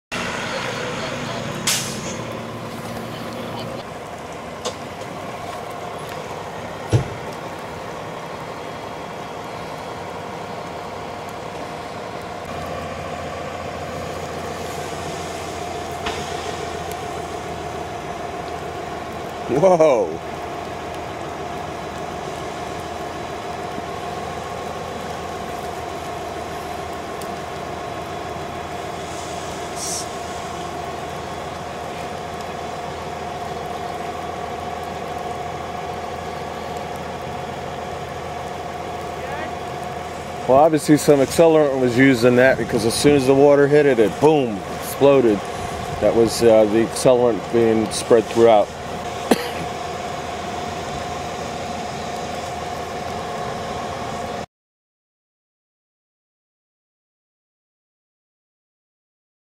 Arson Suspected In Porta Potty Fire--Firefighters Have Nothing To Go On....
Arson suspect in porta-potty fire at Tunxis Hill Monday night around 11pm.  Firefighters have nothing to go on.